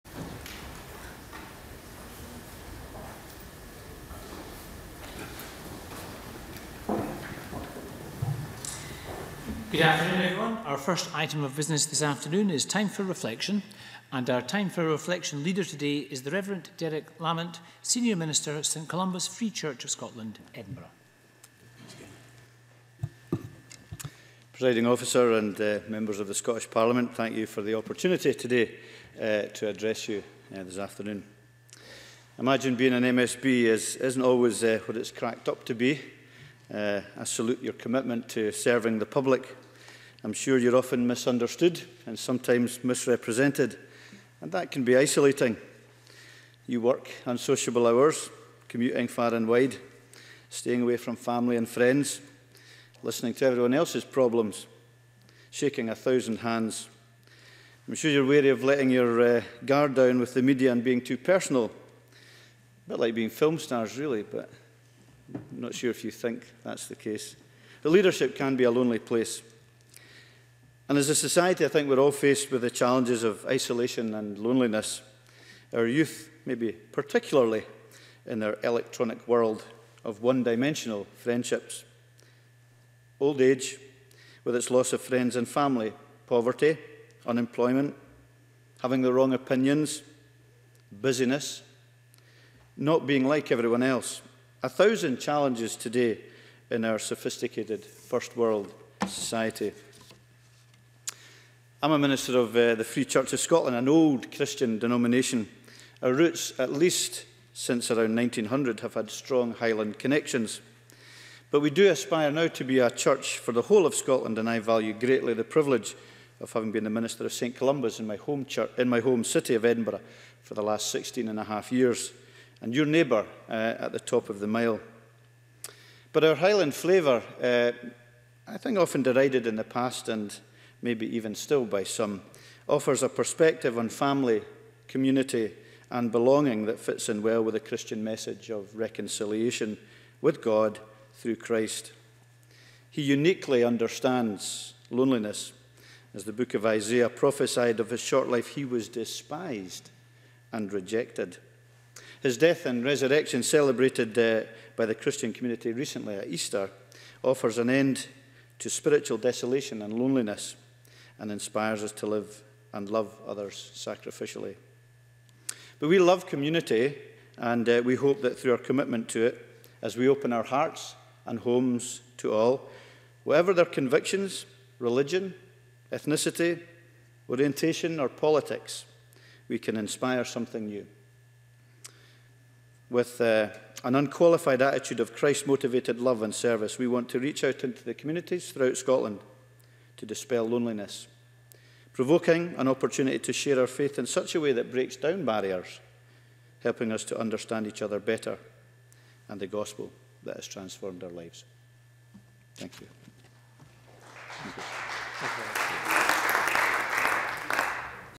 Time for Reflection - Scottish Parliament: 25th April 2017
Reverend Derek Lamont, Senior Minister, St Columba’s Free Church of Scotland, Edinburgh.